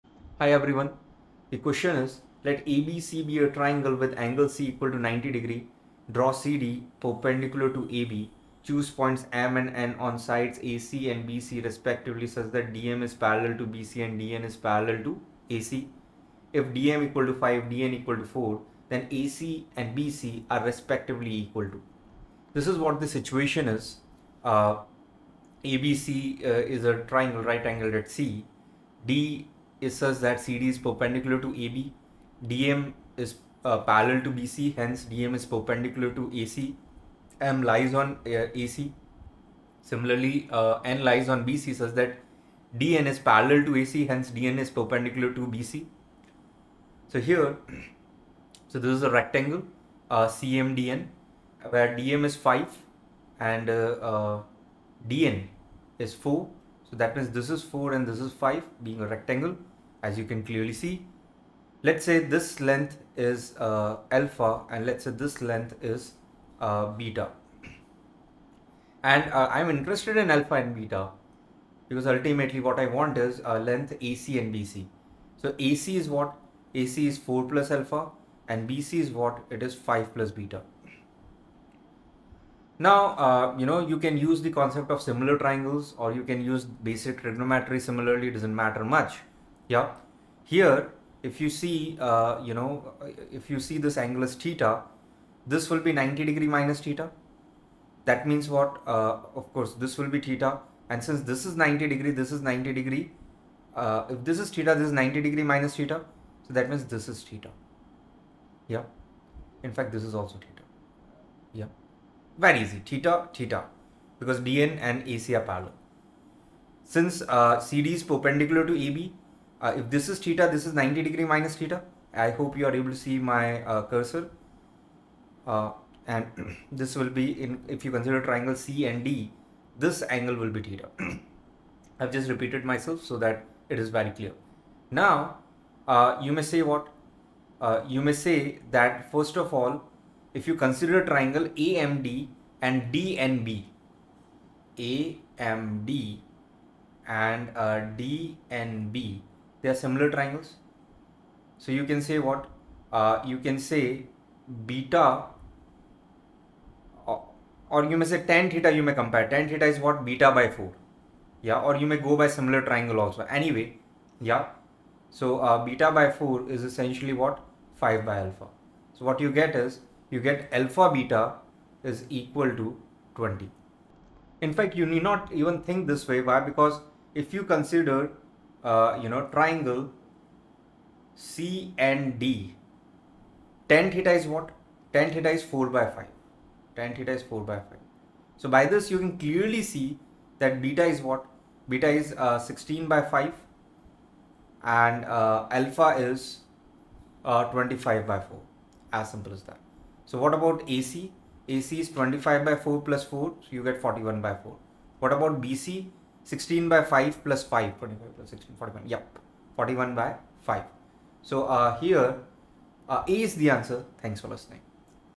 KVPY 2017 SA Nov 5 Mathematics Part I Question 12 Answer A Geometry Triangle Shubham Jhanwar
✅ Session PDF for "KVPY 2017 SA Nov 5 Mathematics Part I Question 12 Answer A Geometry Triangle Shubham Jhanwar" ➡️
➡️Let ABC be a triangle with angle C = 90 degree. Draw CD perpendicular to AB Choose points M and N on sides AC and BC respectively such that DM is parallel to BC and DN is parallel to AC. If DM=5, DN=4 then AC and BC are respectively equal to